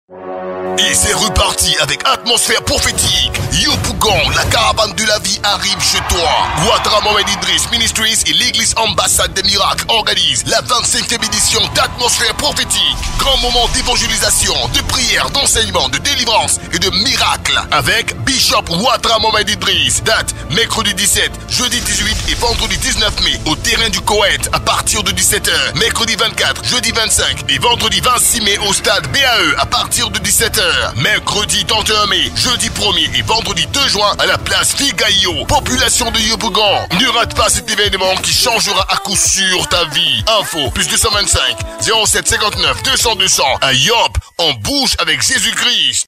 🔥𝐀𝐓𝐌𝐎𝐒𝐏𝐇𝐄𝐑𝐄 𝐏𝐑𝐎𝐏𝐇𝐄𝐓𝐈𝐐𝐔𝐄 𝐘𝐎𝐏𝐎𝐔𝐆𝐎𝐍 𝟐𝟎𝟐𝟑
👉𝐀 𝐘𝐨𝐩, 𝐨𝐧 𝐛𝐨𝐮𝐠𝐞 𝐚𝐯𝐞𝐜 𝐉𝐞́𝐬𝐮𝐬-𝐂𝐡𝐫𝐢𝐬𝐭.
Dates :
🔴Mercredi 17, Jeudi 18 et Vendredi 19 Mai  𝐚𝐮 𝐭𝐞𝐫𝐫𝐚𝐢𝐧 𝐝𝐮 𝐊𝐎𝐖𝐄𝐈𝐓 à partir de 17h00.
🔴Mercredi 24, Jeudi 25 et Vendredi 26 Mai 𝐚𝐮 𝐬𝐭𝐚𝐝𝐞 𝐁𝐀𝐄 à partir de 17h00.
🔴Mercredi 31 Mai, Jeudi 01 et Vendredi 02 Juin 𝐚̀ 𝐥𝐚 𝐩𝐥𝐚𝐜𝐞 𝐅𝐢𝐜𝐠𝐚𝐲𝐨 à partir de 17h00.
Ne rate pas ces différents événements, Jésus-Christ te réserve de grandes choses.